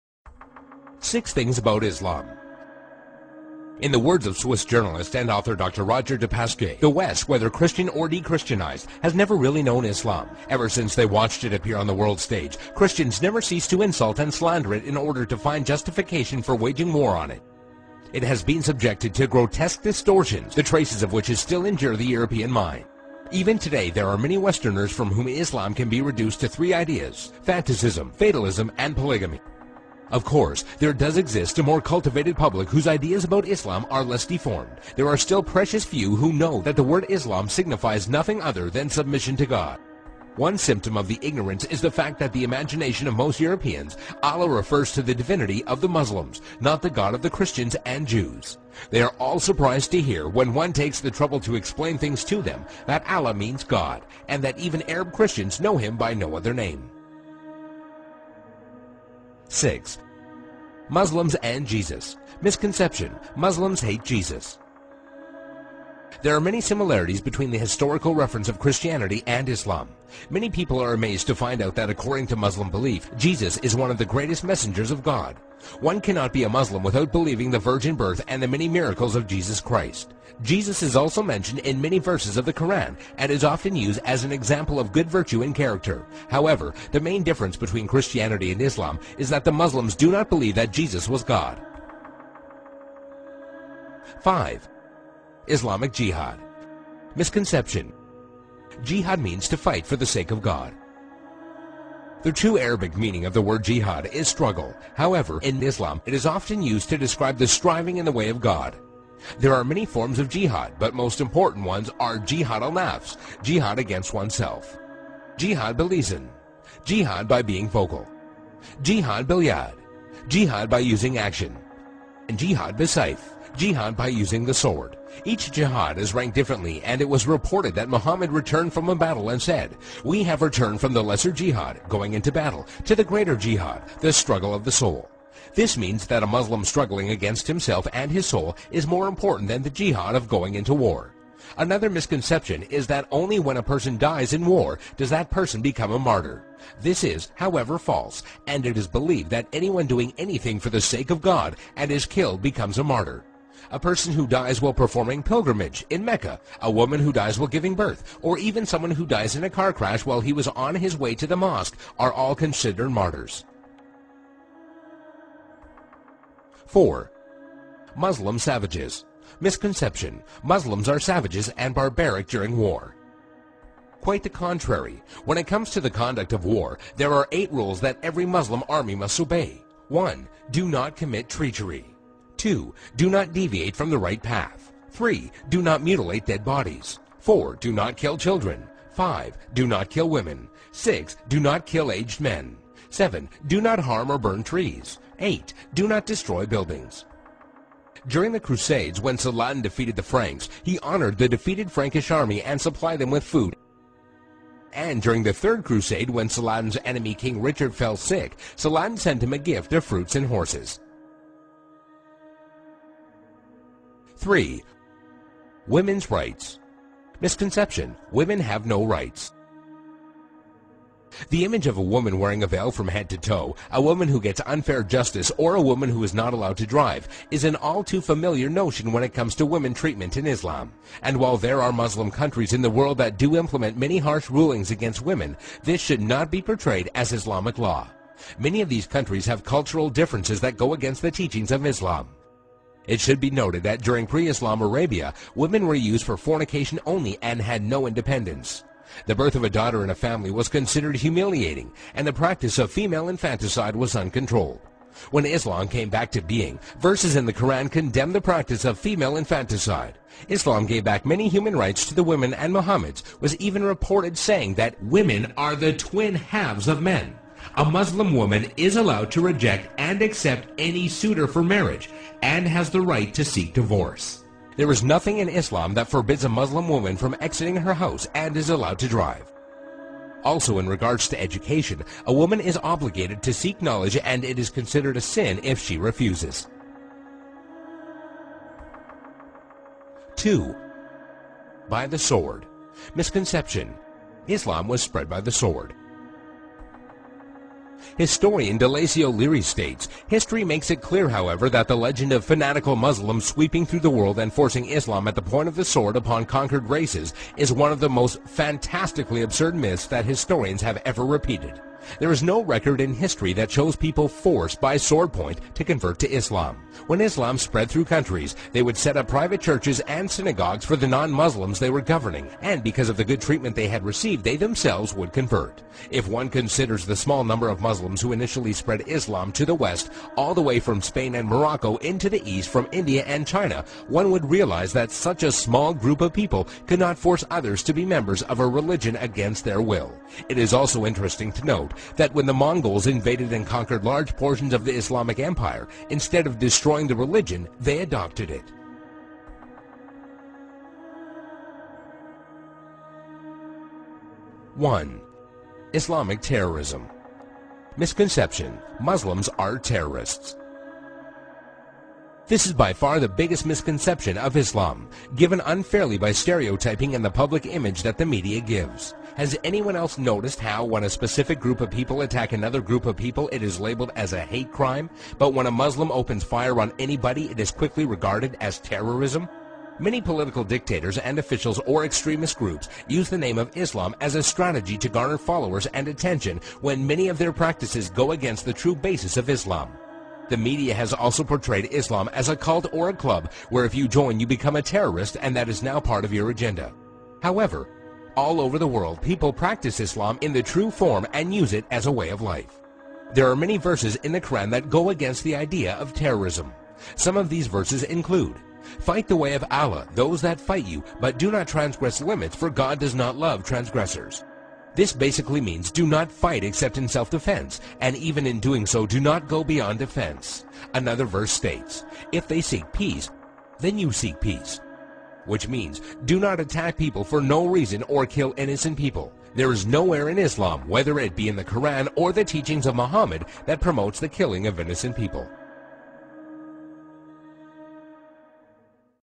TOP 6 MISCONCEPTIONS ABOUT ISLAM
.oO  بسم الله الرحمن الرحیم  Oo.

In the words of Swiss journalist and author, Roger Du Pasquier "The West, whether Christian or dechristianised, has never really known Islam. Ever since they watched it appear on the world stage, Christians never ceased to insult and slander it in order to find justification for waging war on it. It has been subjected to grotesque distortions the traces of which still endure in the European mind. Even today there are many Westerners for whom Islam can be reduced to three ideas: fanaticism, fatalism and polygamy. Of course, there does exist a more cultivated public whose ideas about Islam are less deformed; there are still precious few who know that the word islam signifies nothing other than 'submission to God'. One symptom of this ignorance is the fact that in the imagination of most Europeans, Allah refers to the divinity of the Muslims, not the God of the Christians and Jews; they are all surprised to hear, when one takes the trouble to explain things to them, that 'Allah' means 'God', and that even Arab Christians know him by no other name. "

The common image of a Muslim is a turbaned dark Arab man with a long beard. However this image is part of the minority of Muslims. Arabs make only 15% of the world's Muslim population. As a matter of fact the Middle East comes in third with East Asia coming in at first (69%) and Africa (27%) coming in at second. Another common misconception is that all Arabs are Muslims. While the vast majority of Arabs are Muslims (75%), there are many other religions that Arabs practice including Christianity and Judaism.

HD | TOP 5 MISCONCEPTIONS ABOUT ISLAM | 5 THINGS YOU DID NOT KNOW ABOUT ISLAM